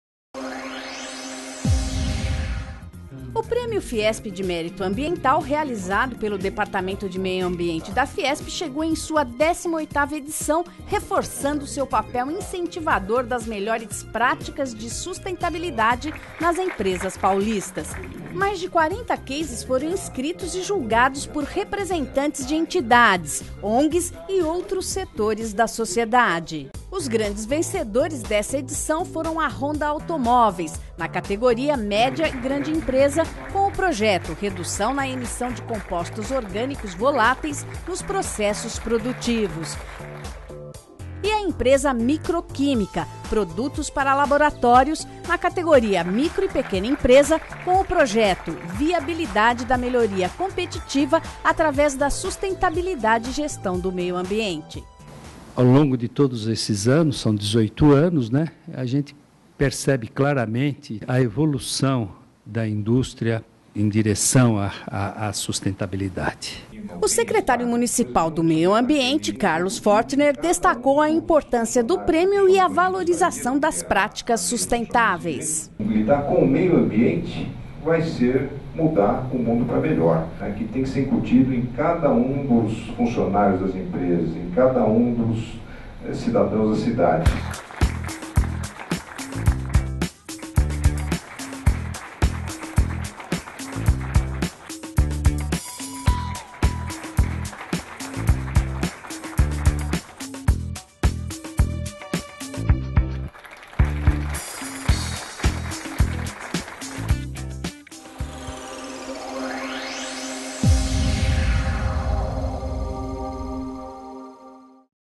18º Prêmio Fiesp de Mérito Ambiental
Em reconhecimento às melhores práticas de sustentabilidade adotadas por empresas instaladas em território paulista, a Federação das Indústrias do Estado de São Paulo (Fiesp), por meio de seu Departamento de Meio Ambiente (DMA), divulgou dia 27/06 os vencedores do 18º Prêmio Fiesp de Mérito Ambiental. A edição deste ano recebeu um total de 41 cases, inscritos nas categorias micro e pequeno porte (até 99 empregados) e médio e grande porte.

Com o projeto "Redução na emissão de Compostos Orgânicos Voláteis (VOC) nos processos produtivos", a Honda Automóveis foi a grande vencedora da categoria médio e grande porte. Na categoria micro e pequena empresa, o primeiro lugar ficou com a empresa Micro-Química Produtos para Laboratórios Ltda, com o projeto "Viabilidade da melhoria competitiva através da sustentabilidade e gestão do meio ambiente".